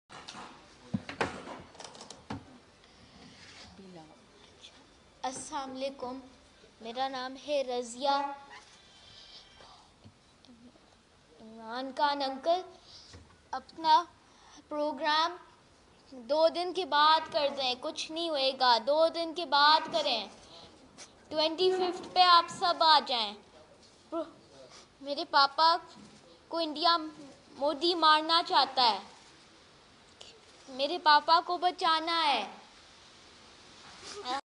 Mishal Malik, wife of Hurriyat leader Yasin Malik, appeals to Imran Khan to postpone
Mishal Malik, wife of Hurriyat leader Yasin Malik, appeals to Imran Khan to postpone the long march for two days.